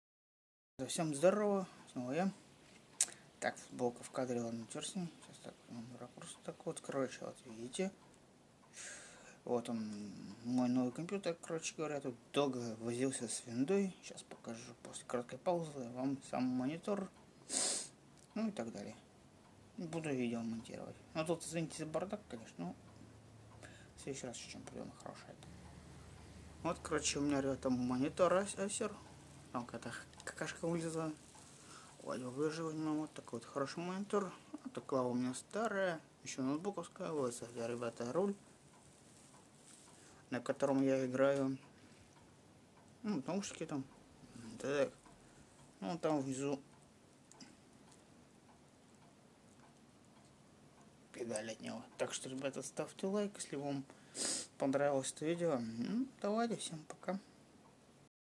новый пк как вам параметры в описании
Процессор Intel Core i5-6500

Видеокарта Asus GeForce GTX 1060 STRIX OC [STRIX-GTX1060-O6G-GAMING]
Код товара: 1068471
 2шт Оперативная память Crucial [CT8G4DFS8213] 8 ГБ

120 ГБ SSD-накопитель Samsung 750 EVO [MZ-750120BW]
Код товара: 1043802
1 ТБ Жесткий диск Seagate 7200 BarraCuda [ST1000DM010]